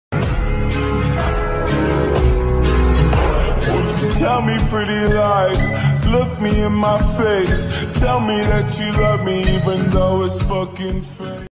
"Tell me pretty lies.."||OC angst|| New Oc??|| Context in desc.
So you might notice the the boy with Brown hair is my OC..

Important Characters:

Brown hair boy: Peka Atsuma (pe-kuh)
blue hair girl: Raia Yunama(rye-yuh)

CONTEXT:
Peka was a quirkless orphan. Just like Deku, people turned on him. including his girlfriend and childhood friend Raia Yunama. Peka broke up with Raia afterwards. they are now Ex lovers and ex friends. He was eventually found By Aizawa who adopted him. He keeps his regular name but is now a student in support course. :)